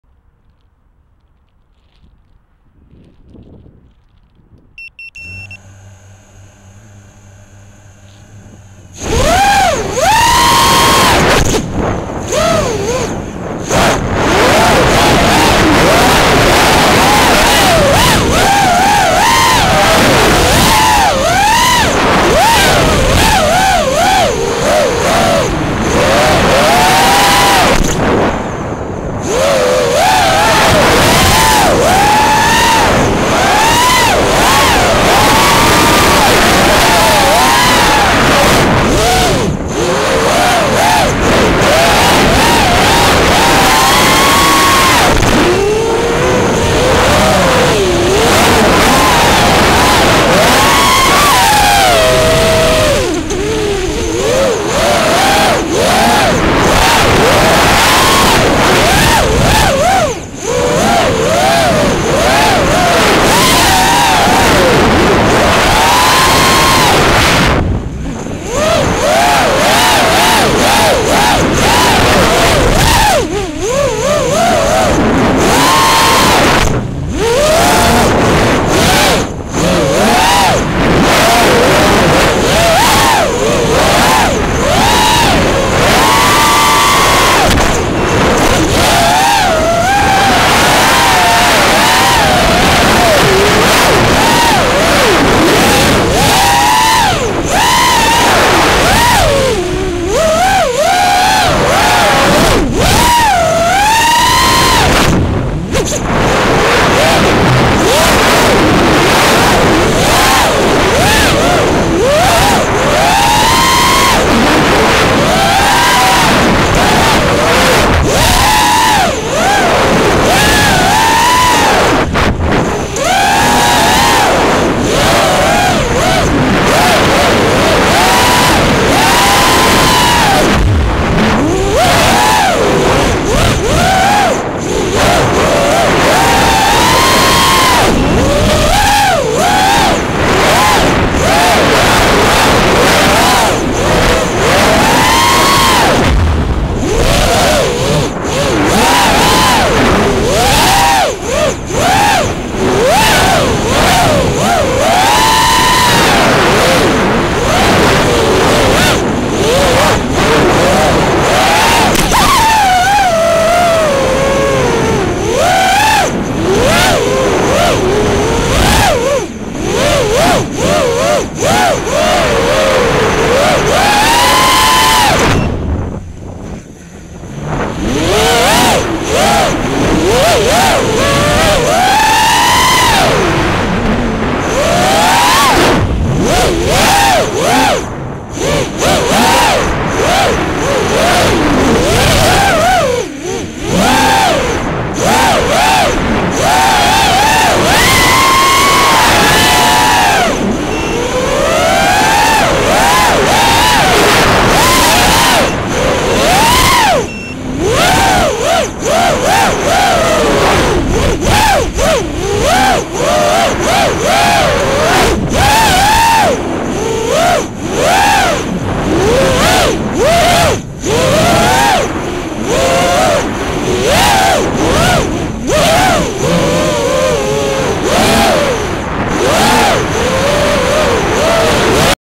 런캠 스플릿 프리스타일?
이번에 조립한 것은 딜레이도 없고 좋음.
헌데 촬영영상 중앙부위에 줄무늬 스크레치 현상 있음. ㅜㅜ